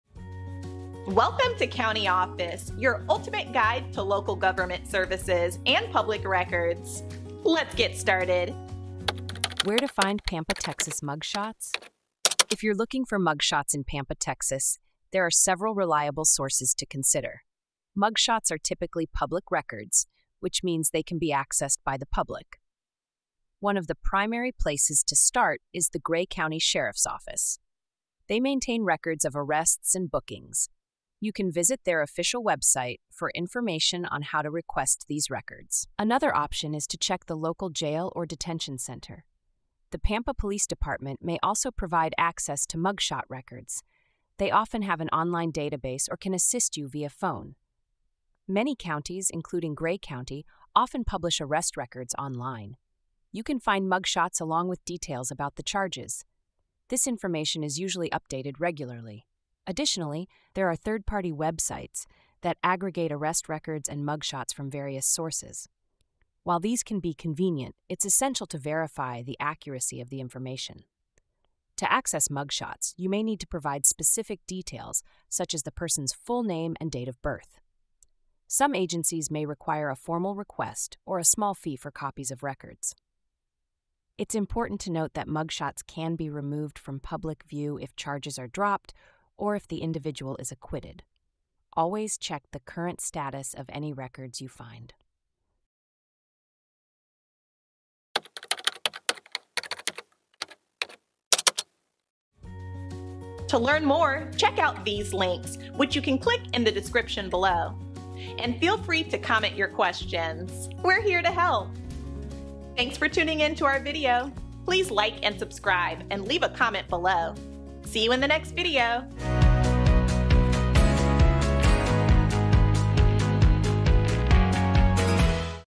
Where To Find Pampa, Texas Mugshots? - CountyOffice.org
Where To Find Pampa, Texas Mugshots? Have you ever wanted to know where to find mugshots in Pampa, Texas? Mugshots are public records that can be accessed through various reliable sources in the area. In this video, we'll guide you on where to look for mugshots, including the Gray County Sheriff's Office, local jails, and the Pampa Police Department. We'll also discuss how you can access mugshot records online and through third-party websites. Learn about the process of requesting mugshots and the importance of verifying the accuracy of the information you find. Stay informed on how mugshots can be removed from public view based on legal outcomes.